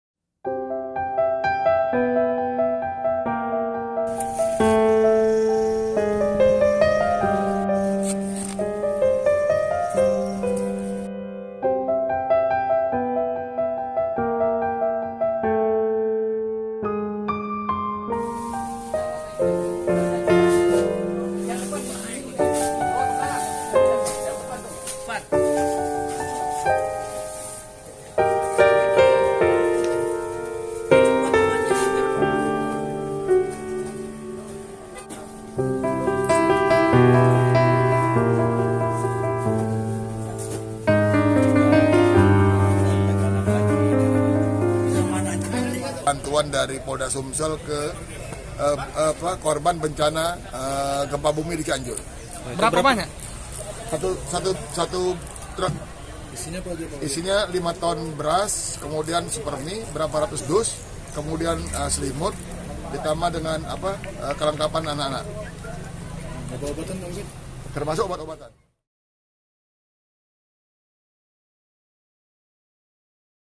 Polda Sumsel Kirim Bantuan Korban Gempa Cianjur
JAKARTA, KOMPAS.TV - Polda Sumatera Selatan salurkan bantuan logistik untuk korban bencana alam gempa bumi di Kabupaten Cianjur, Jawa Barat, pada Rabu (23/11/2022).

Bantuan itu terdiri dari lima ton beras, paket makanan siap saji, selimut dan perlengkapan anak-anak seperti pakaian serta obat-obatan.

Baca Juga Relawan Gempa Cianjur Blusukan Cari Lokasi yang Belum Dapat Bantuan di

Bantuan itu dikirim menggunakan satu unit truk boks angkutan barang dengan pengawalan dari Direktorat Lalu Lintas Polda Sumsel.